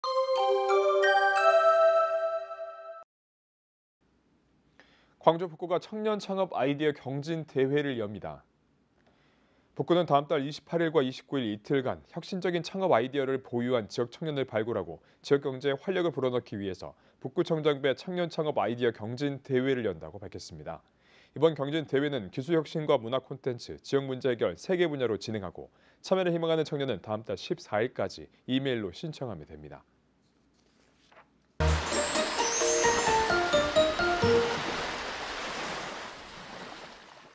[광주뉴스] 광주 북구, '청년창업 아이디어 경진대회' 개최
■CMB뉴스

광주 북구가 청년창업 아이디어 경진대회를 엽니다.

북구는 다음달 28일과 29일 이틀간,
혁신적인 창업 아이디어를 보유한 지역 청년을 발굴하고
지역경제에 활력을 불어넣기 위해
´북구청장배 청년창업 아이디어 경진대회´를 연다고 밝혔습니다.

이번 경진대회는
기술 혁신과 문화콘텐츠, 지역문제 해결 3개 분야로 진행되고,
참여를 희망하는 청년은 다음달 14일까지 이메일로 신청하면 됩니다.